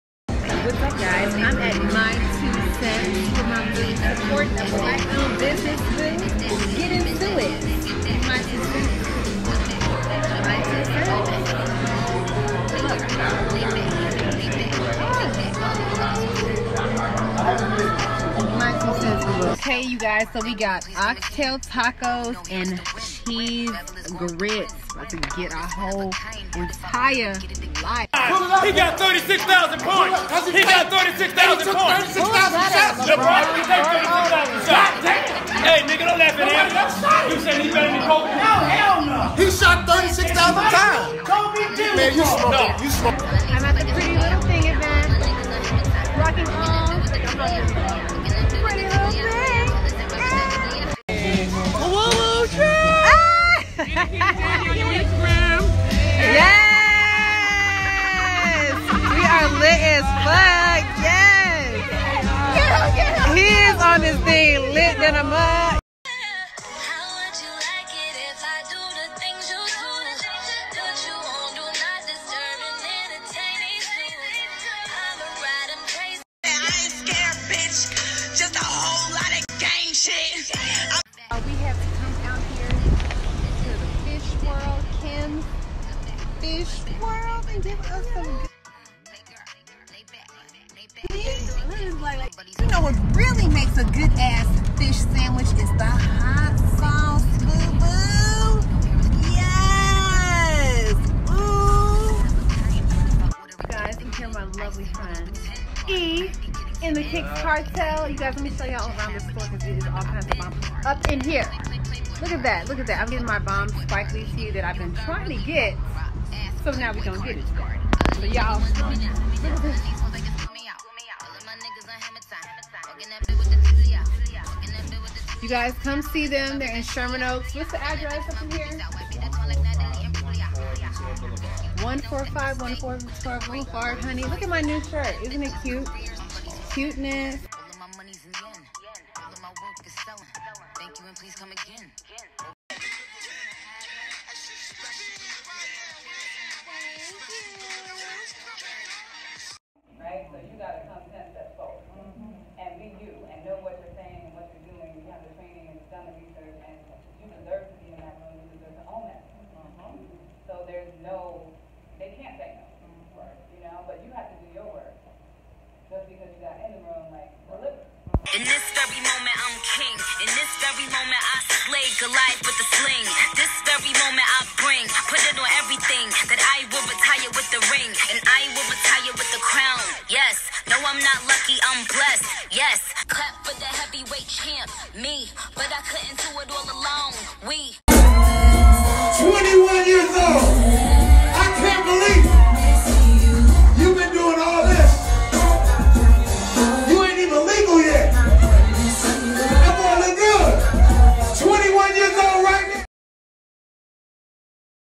KATADVENTURES VLOG #2 | Beauty - Sexy Dresses - Celebs - Hella’ Food - Abs & Entrepreneurs
Sharing a few of my wild adventures & yummy eats from the last few months!